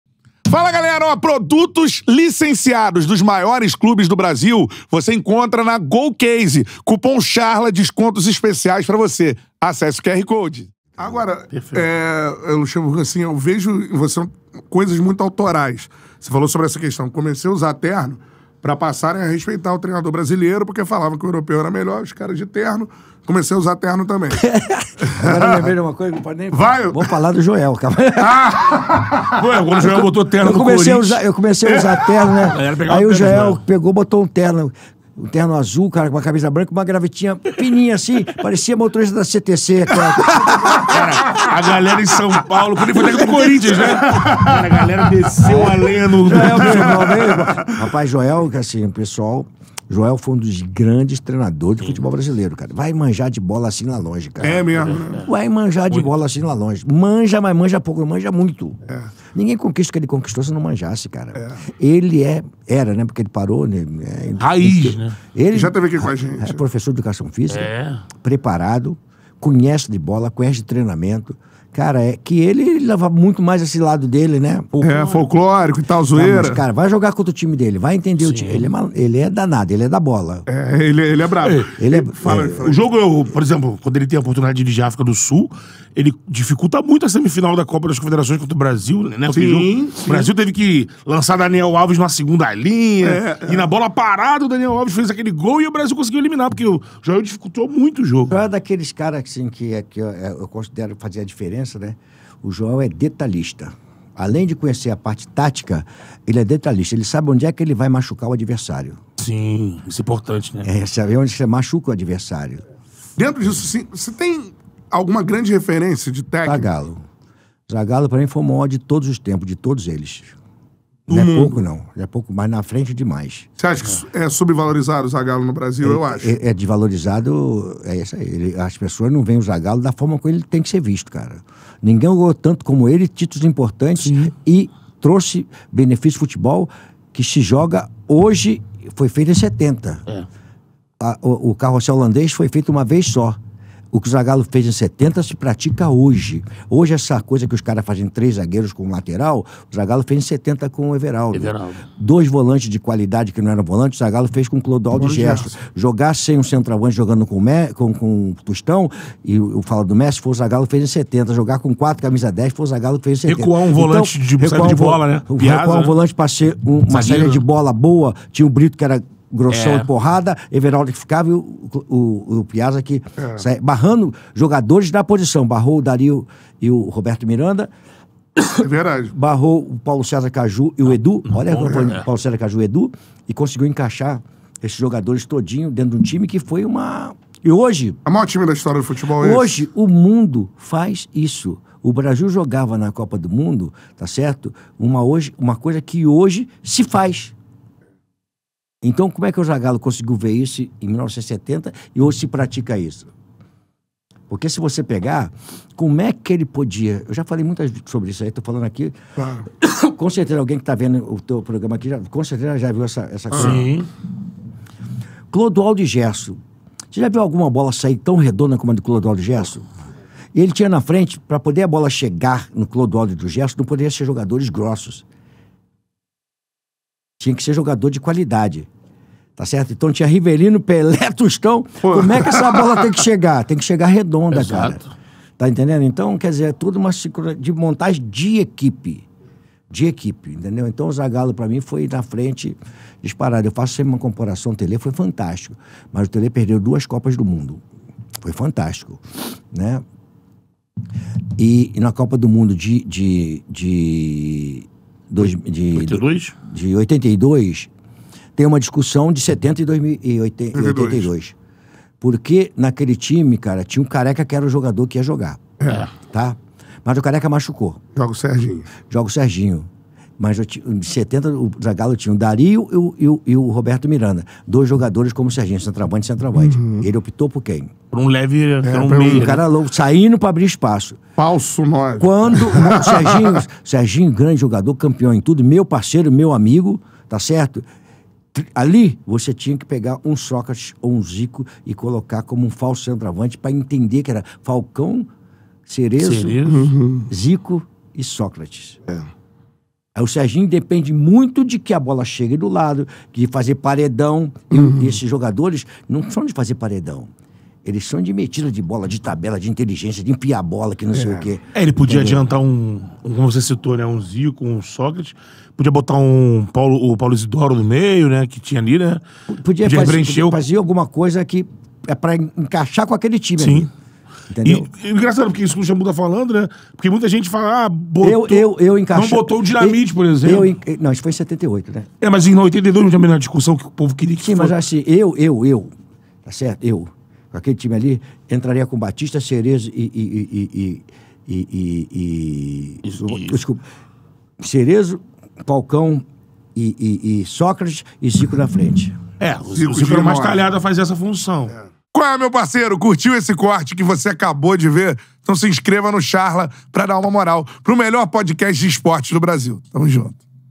O QUE O ZAGALLO REPRESENTA PRO FUTEBOL MUNDIAL?
O Charla Podcast #485 recebe Vanderlei Luxemburgo, um dos maiores técnicos da história do Brasil.
Acesse o site da GOCASE e utilize o cupom CHARLA para garantir descontos em produtos licenciados dos maiores clubes do Brasil.